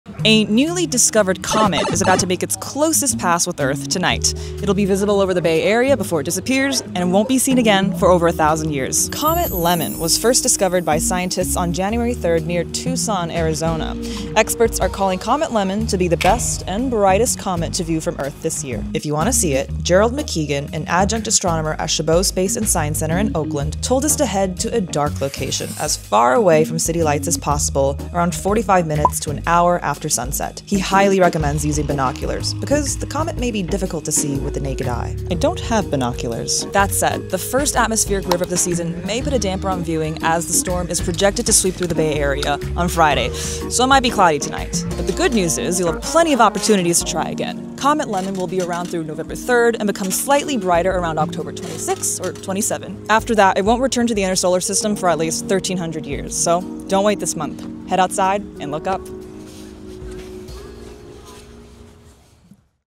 New comet alert!!!
A newly discovered comet is about to make its closest pass to Earth on Tuesday night. It should be visible over the Bay Area before it disappears — and won’t be seen again for more than a thousand years